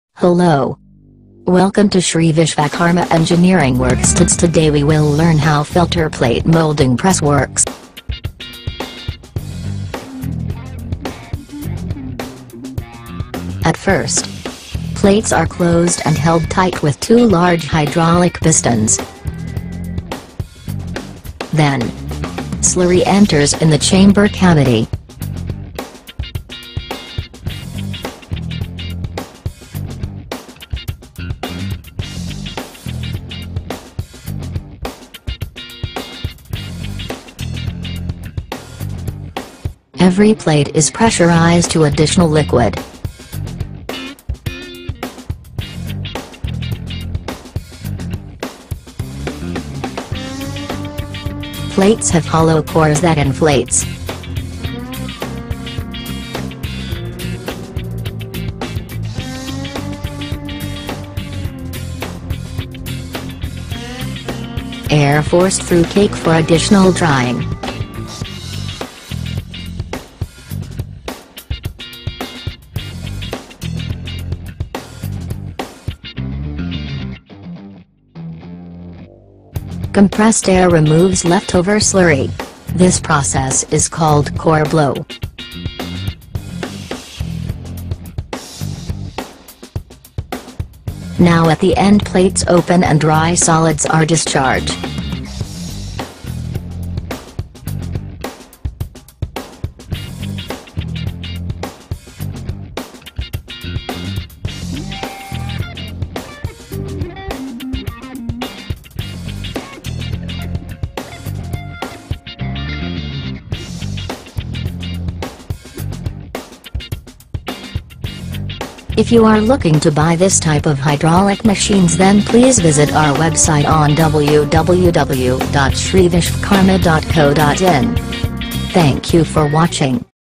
Filter plate molding Hydraulic press Working
This video is informative video which shows how filter plate Hydraulic press works. Shree Vishwakarma engineering works is also the manufacturer and supplier of filter plate Hydraulic press.